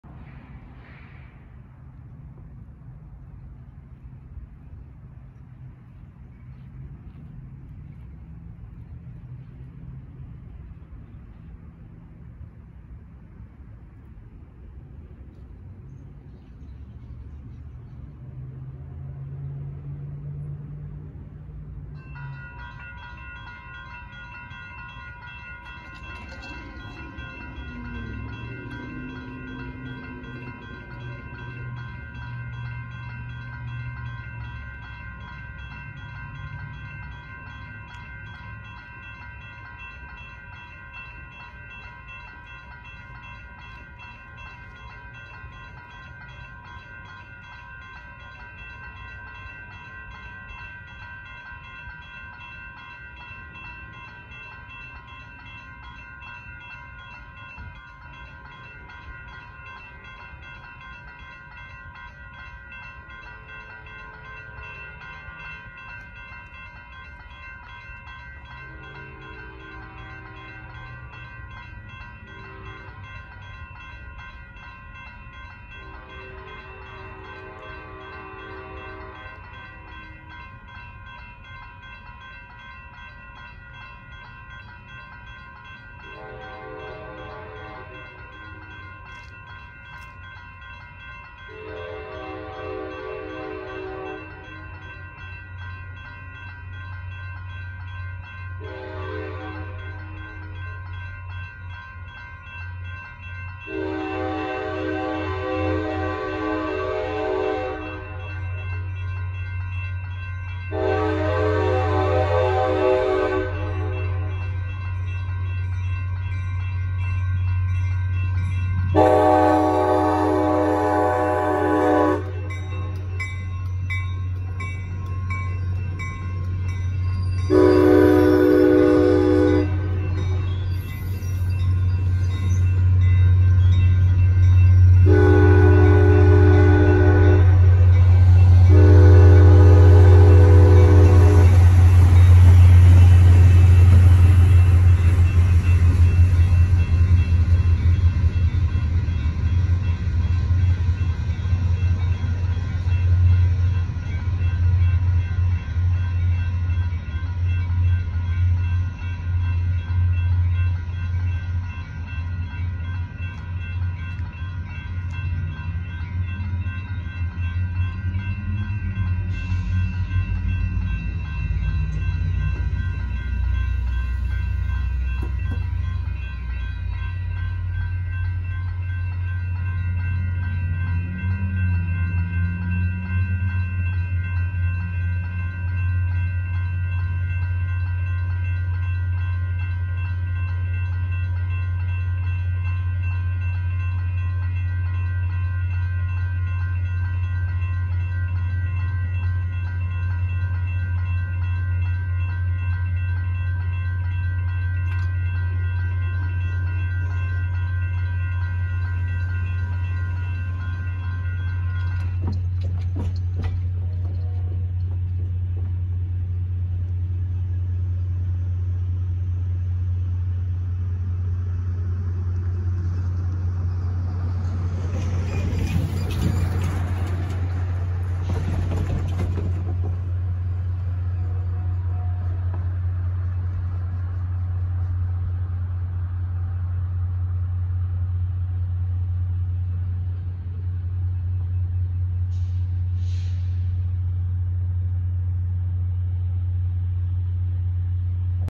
Altamont Commuter Express 3304 #08 North, E. Main St. Railroad Crossing, Stockton CA
My last catch was ACE #08 arriving into the Stockton ACE Station. I setup at East Main Street to catch them. This is my first time catching ACE at this crossing after avoiding them here for a long time because the gates kept getting stuck in the down position until the ACE departed the station. As of lately that hasn’t been happening that much. This crossing is a mix install with the gateless signals being a SPRR 1980s install with changes to their bells and lights and the gated masts being a UPRR mid to late 2010s install.

Train in the video:
Altamont Commuter Express -ACE - #04: ACEX 3105 EMD F40PH-3C, Lead CabCar 3304 ACEX

DingerFan#16